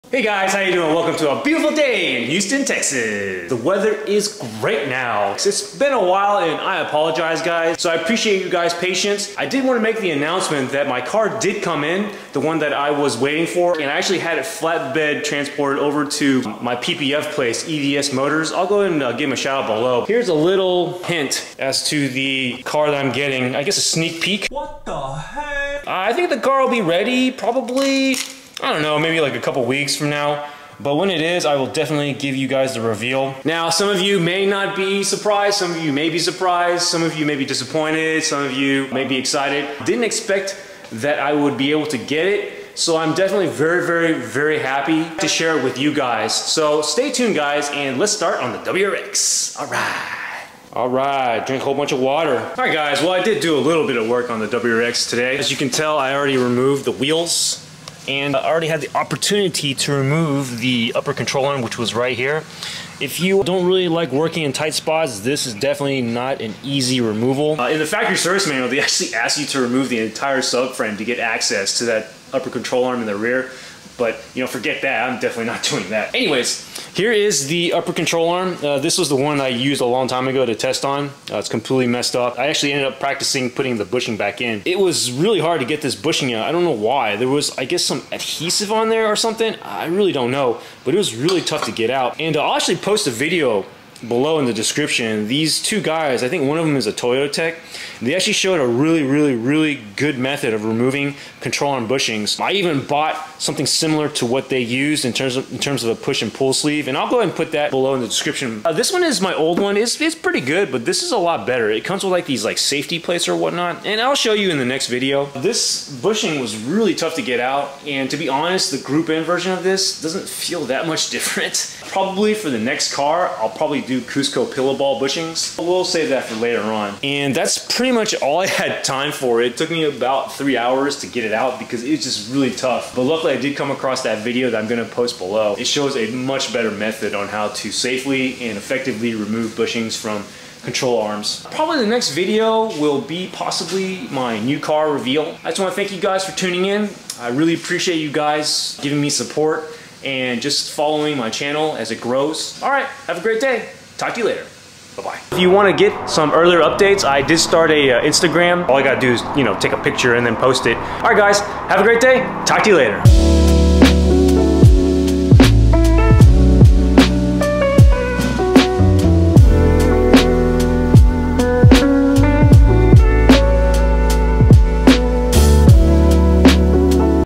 2017 WRX got Group N work done! Keys to my new........??
2017 Subaru WRX

@EVSMotors  Paint Protection Film Crew in Houston Texas

@briantoolive Watched this video to see how Rear Upper Control Arm is removed without lowering the subframe: